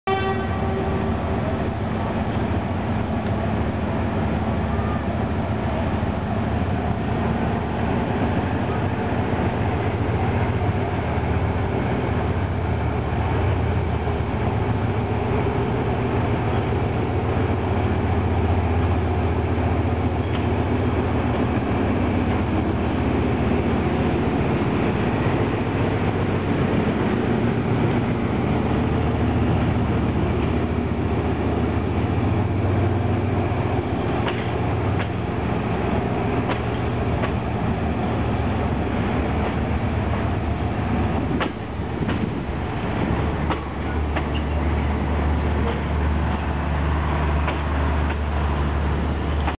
SRT: Departure of 7.30pm express train to Nakhon Sri Thammarat.
This is a short movie of the departure of the 7.30pm express train to Nakhon Sri Thammarat. Loco number GEA 4549. Loco type general electric 2nd generation. Built in 1995. This type of loco are the most powerful that SRT operate. This train is the longest passenger train that SRT regularly operates with at least 22 carriages.